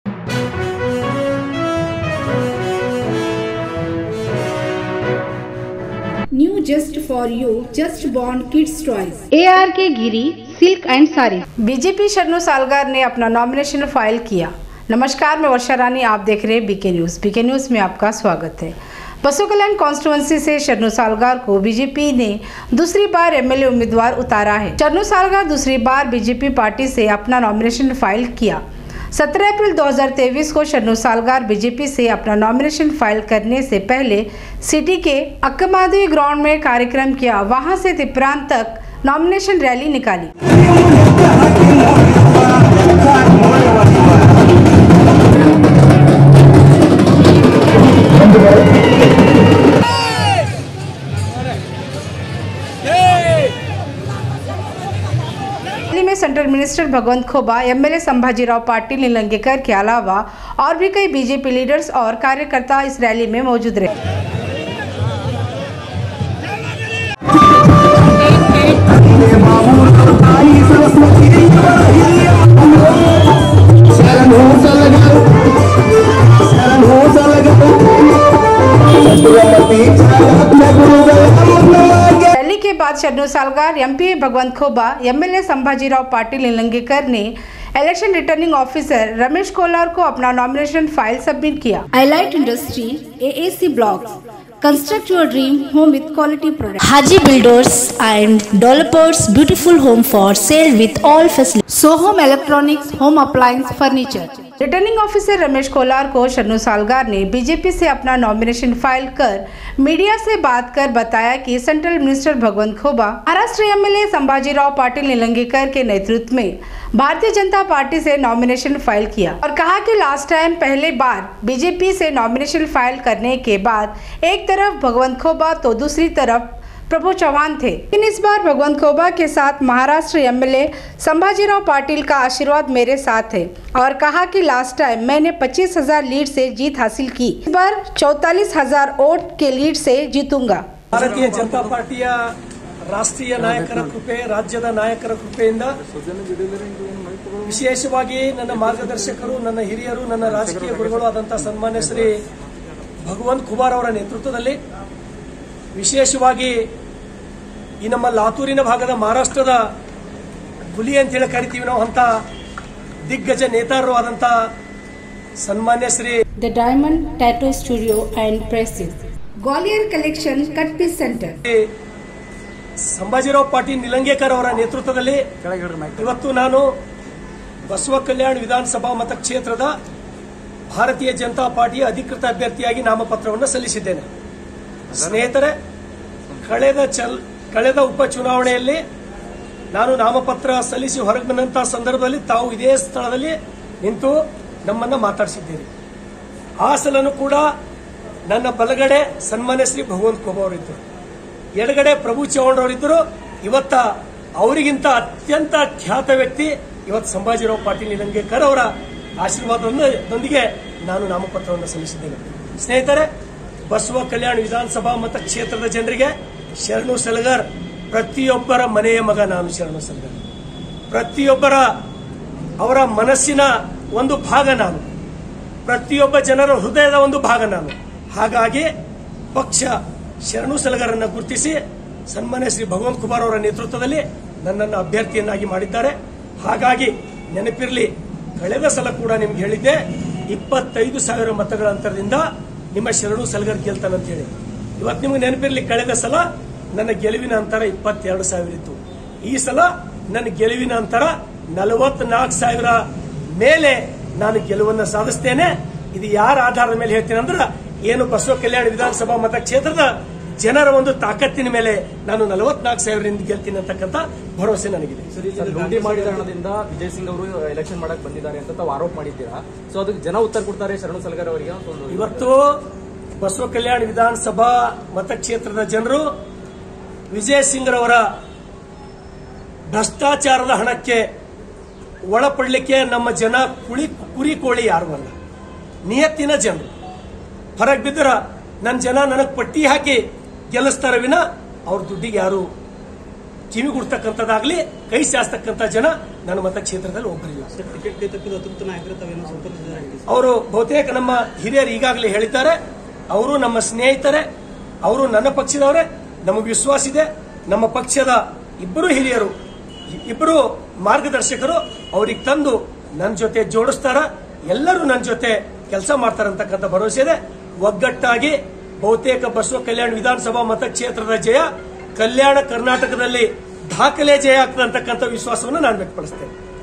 Sharanu Salgar ne BJP se file Nomination kiya/BK NEWS BASAVAKALYAN 2843
Sharanu Salgar ne kiya BJP se Nomination File